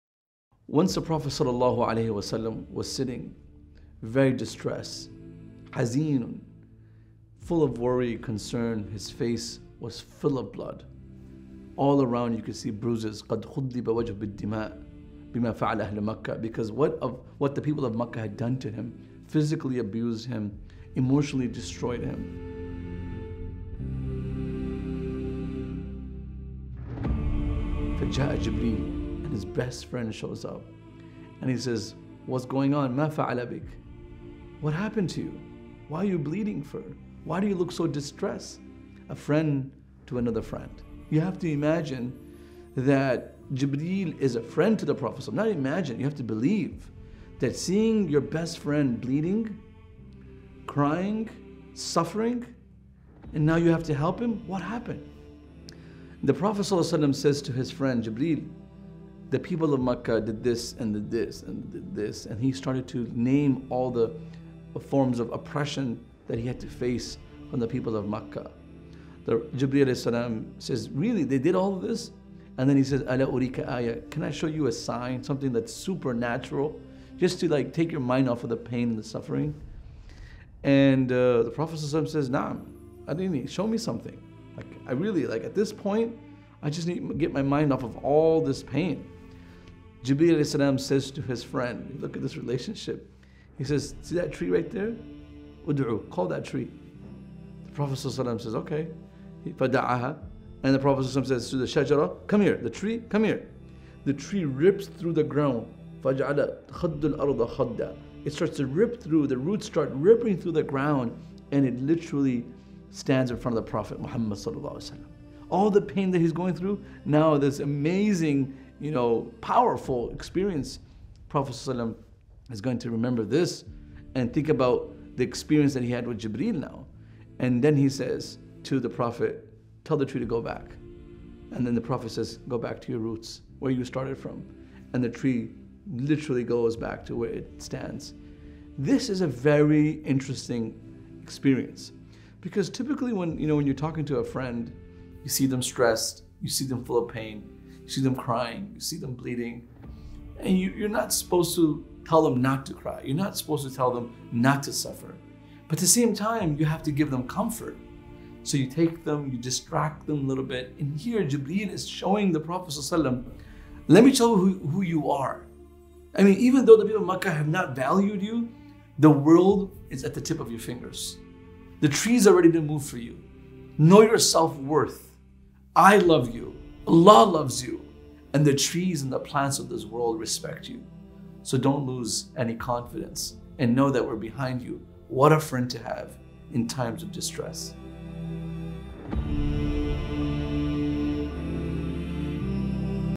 Ep. 04 - A Comfort of a Friend | Jibril (AS) with the Prophet (ﷺ)
The Prophet SAW wasn't having the easiest time with the Quraysh early in his prophethood. Jibril AS was a friend that comforted him with a sign that showed him his self-worth, in a time where he SAW did not feel encouraged. Just as friends do, they pick you up when you're down.